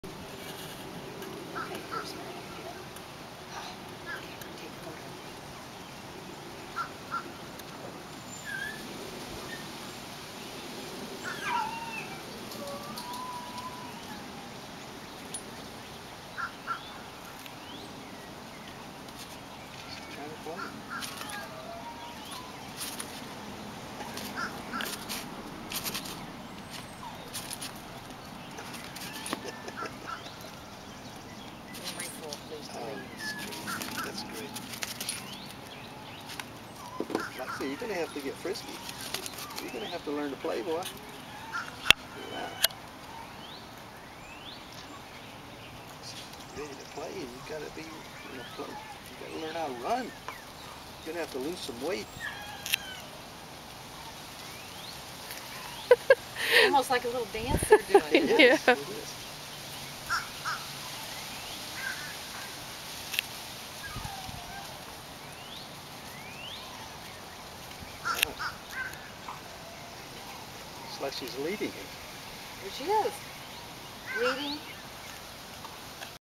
Jozie meets Moxie
Potential adoptors come for a visit. Jozie took to their dog Moxie better than we expected. Jozie was adopted and now lives in Florida.